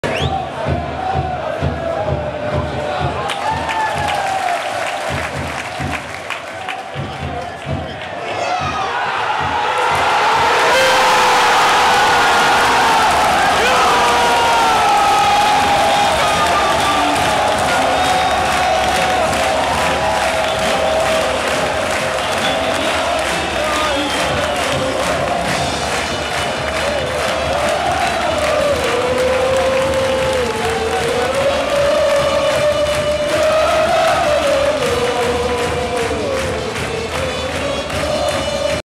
Roda JC - NAC Breda 4-3, doelpunt / goal Malki 1-0
Malki maakt het eerste doelpunt van de avond.